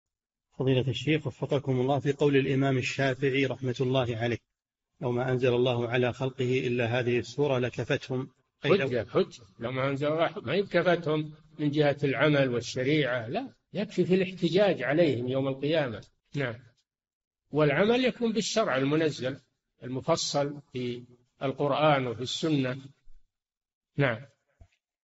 [4542- 4717] ما المراد بقول الشافعي: لوما أنزل الله على خلقه إلا هذه السورة لكفتهم؟ - صالح الفوزان
"[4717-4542] في قول الإمام الشّافعي رحمة الله عليه: لو ما أنزل الله على خلقه إلا هذه السورة لكفتهم؟
الجَوَابُ: حجة، حجة، لو ما أنزل الله حجة، ما هي كفتهم من جهة العمل والشريعة لا، يكفي في الاحتجاج عليهم يوم القيامة، والعمل يكون بالشرع المنزل المفصل في القرآن وفي السُّنة.

للاشتراك في قناة الشيخ صالح الفوزان
فتاوى المجالس في تفسير المفصل

للاستفسار والإقتراحات والدعم
رئيس الفريق
لمشروع كبار العلماء